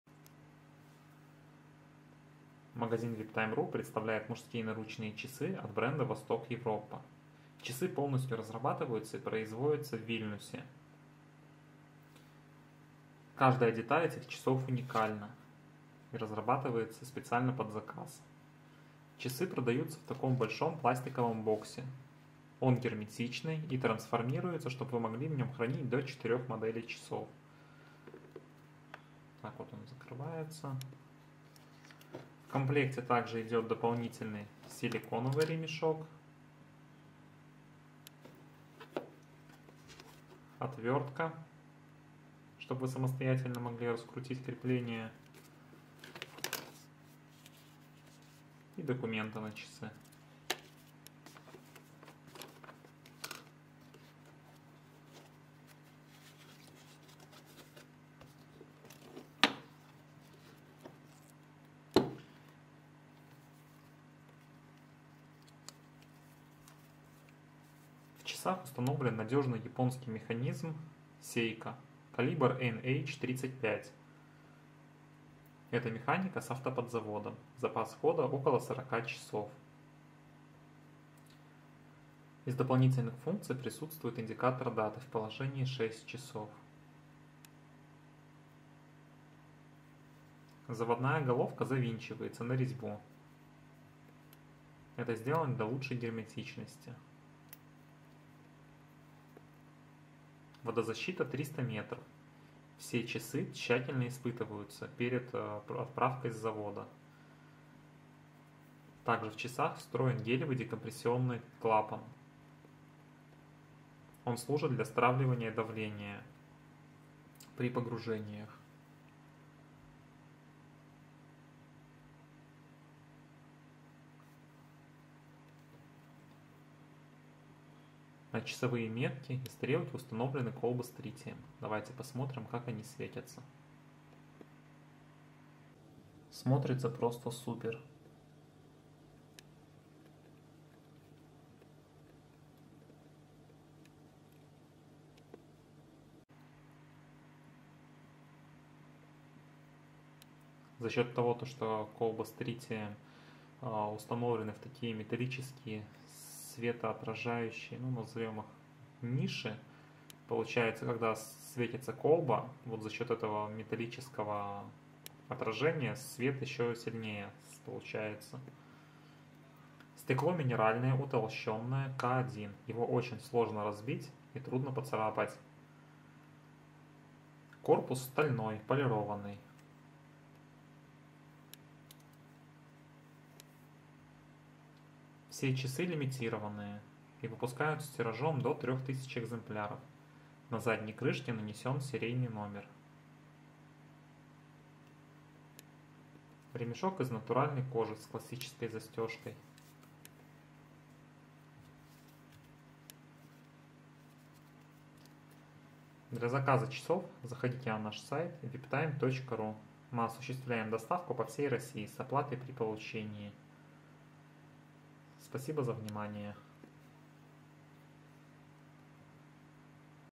💥💥💥Восток Европа Энергия NH35A/575A279
Часы из нержавеющей стали, круглой формы и завинчивающейся головкой. Циферблат синего цвета, с накладными знаками. Стрелки оригинальной формы. Индексы циферблата и стрелки выполнены из светящихся трубок Trigalight ® Micro. Часы имеют календарь числа месяца. Часы укомплектованы кожаным ремнем и индивидуальной упаковкой.

Калибр механизма Seiko NH35A (производится в Японии)

Стекло минеральное упрочненное К1

В комплекте с часами кожаный и силиконовый ремень с отверткой и пластиковый чемодан для хранения 4-х моделей часов.